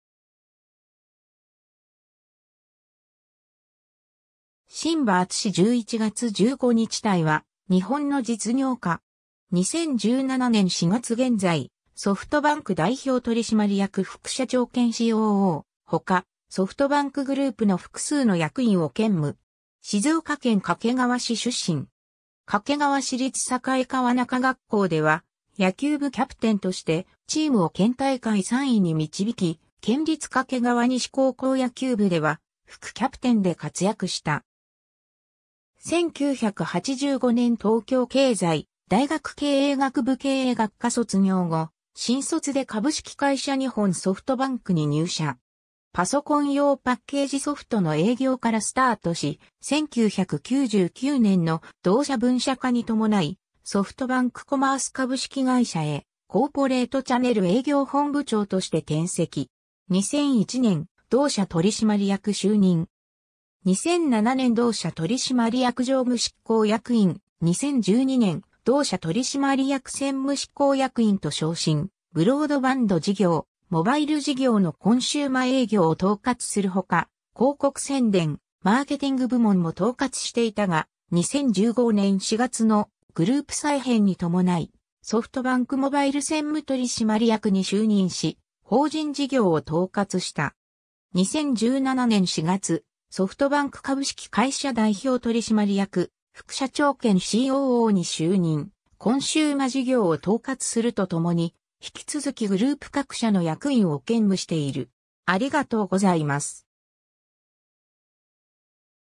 榛葉淳
榛葉 淳（しんば じゅん、1962年（昭和37年）11月15日 - ）は、日本の実業家。
2017年4月現在、ソフトバンク代表取締役副社長兼COO他、ソフトバンクグループの複数の役員を兼務。
 静岡県掛川市出身。
掛川市立栄川中学校では野球部キャプテンとしてチームを県大会3位に導き、県立掛川西高校野球部では副キャプテンで活躍した。
1985年 東京経済大学経営学部経営学科卒業後、新卒で株式会社日本ソフトバンク（現ソフトバンク株式会社）に入社。
パソコン用パッケージソフトの営業からスタートし、1999年の同社分社化に伴いソフトバンク・コマース株式会社（ソフトバンクBB株式会社、ソフトバンクモバイル株式会社、現ソフトバンク株式会社）へコーポレートチャネル営業本部長として転籍。
 2001年、同社取締役就任。
2007年 同社取締役常務執行役員、2012年 同社取締役専務執行役員と昇進、ブロードバンド事業、モバイル事業のコンシューマ営業を統括するほか、広告宣伝、マーケティング部門も統括していたが、2015年4月のグループ再編に伴いソフトバンクモバイル専務取締役に就任し、法人事業を統括した。
 2017年4月、ソフトバンク株式会社代表取締役副社長兼COOに就任、コンシューマ事業を統括すると共に、引き続きグループ各社の役員を兼務している。